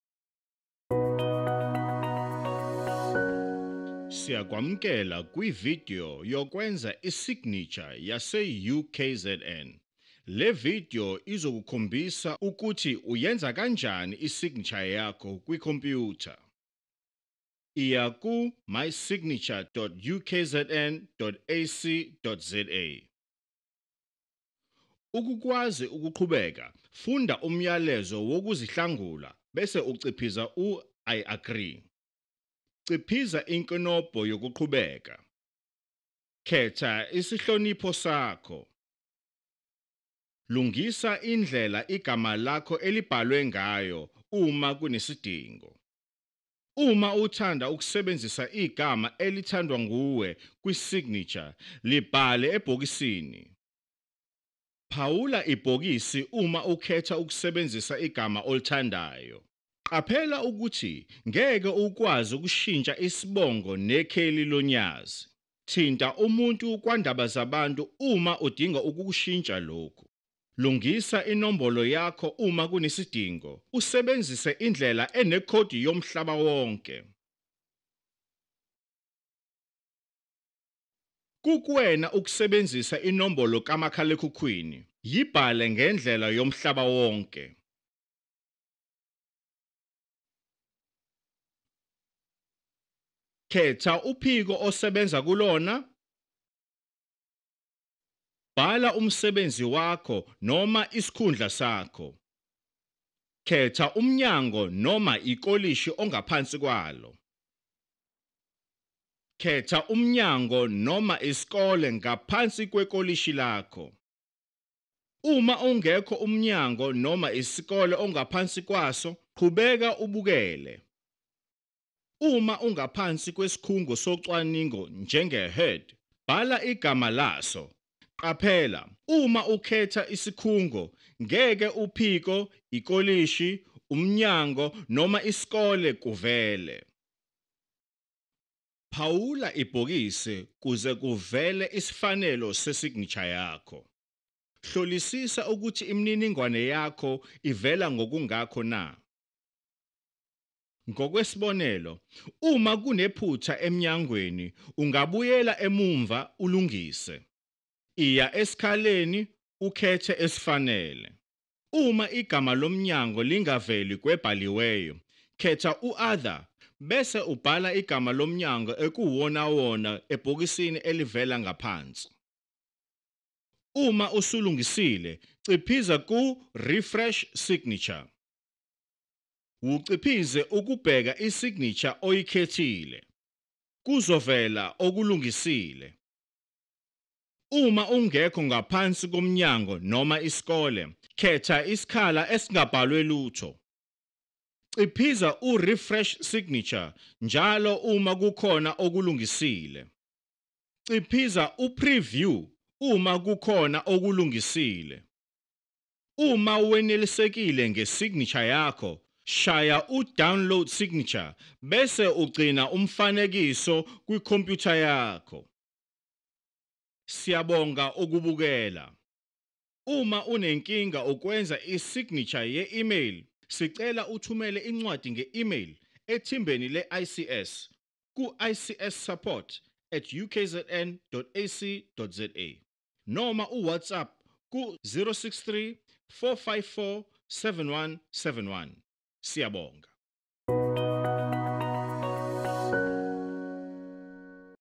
How to generate your Email Signature – isiZulu Version
UKZN signature instruction video on how to generate your Email Signature. This is the isiZulu version.